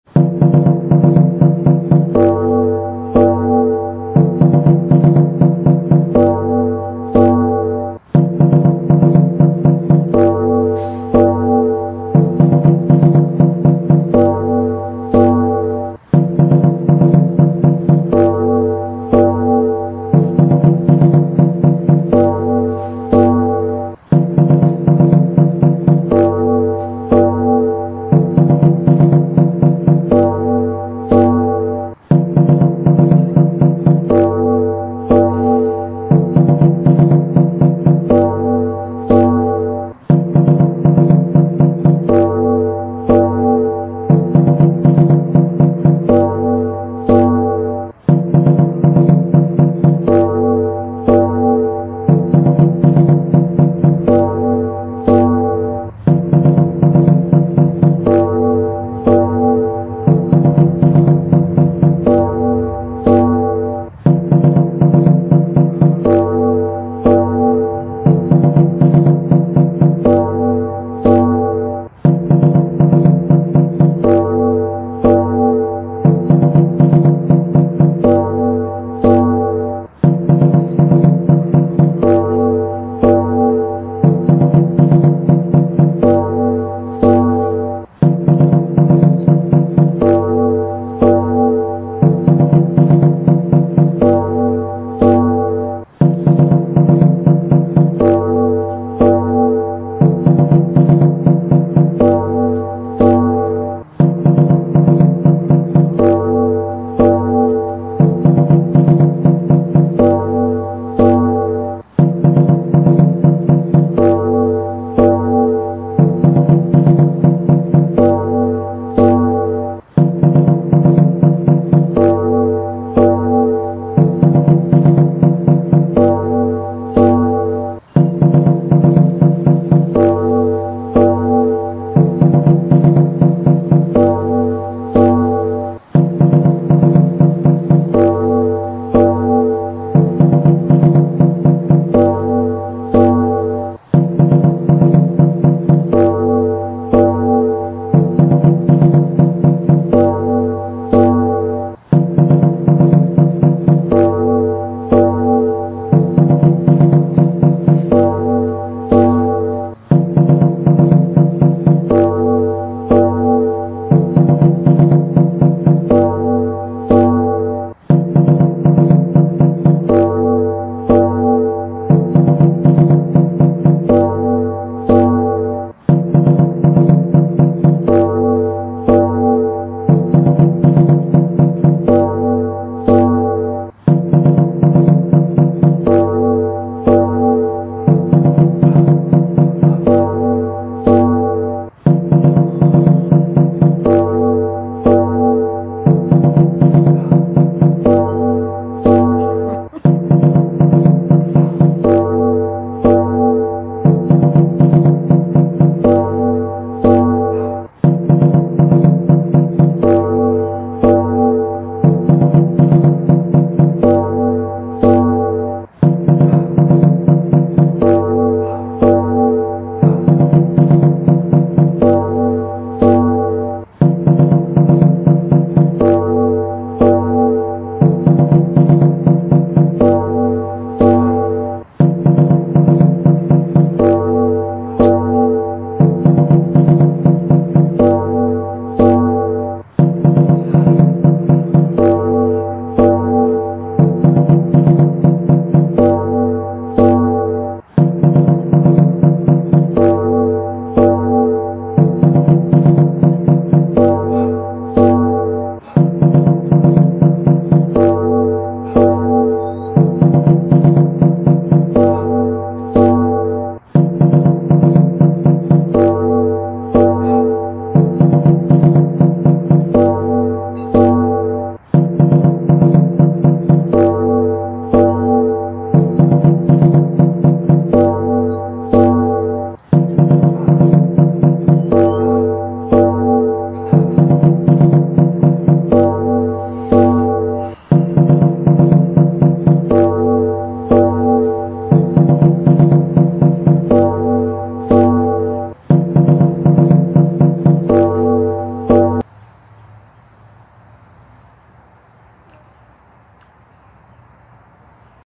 Hair Spell
Playing with my hair, while sending my intentions. To Iban Eshu.